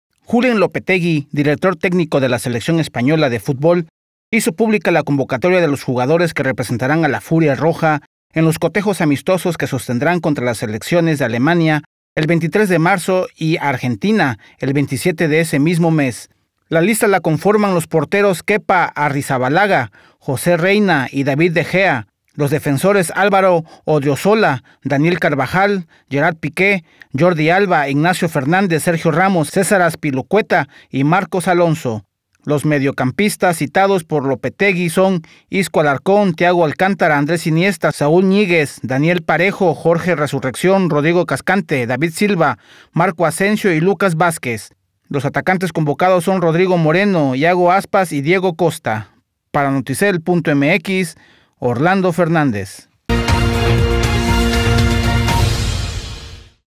España jugará amistosos
La Selección vela armas para enfrentar a Alemania y Argentina